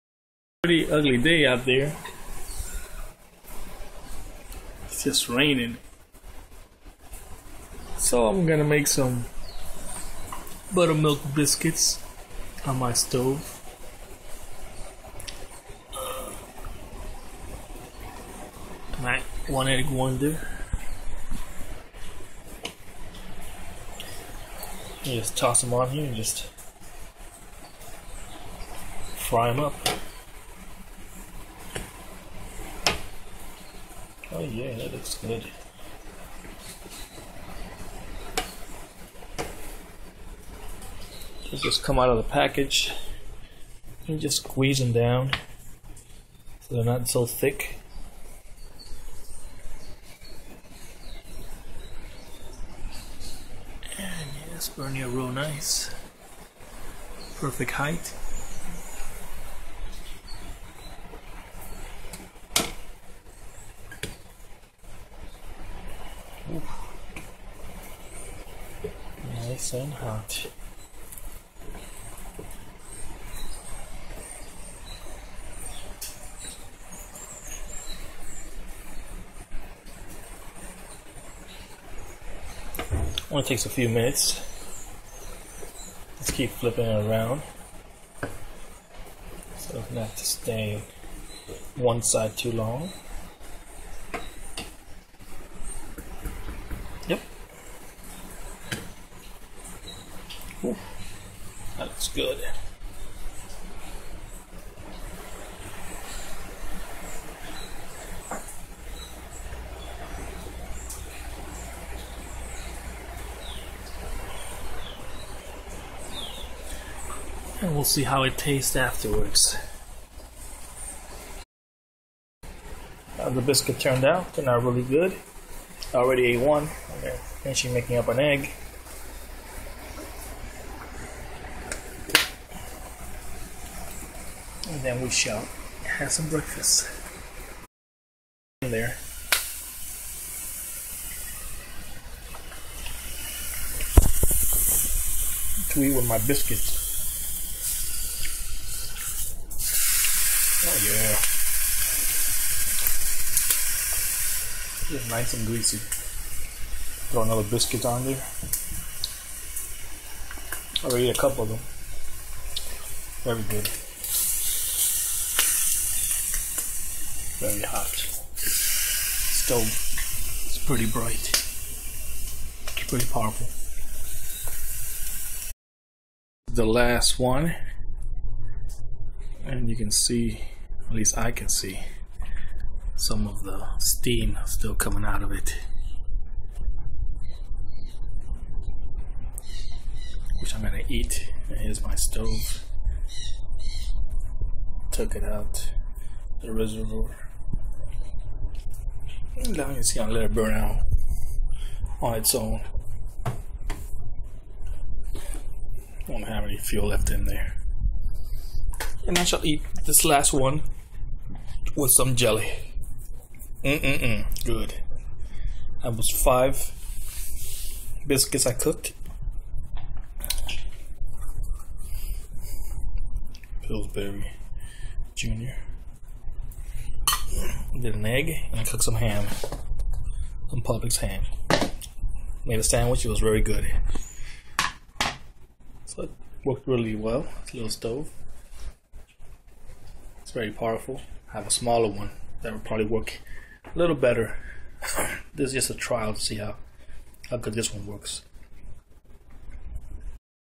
Breakfast on a rainy day
Trying out my carbon fiber stove i made to make breakfast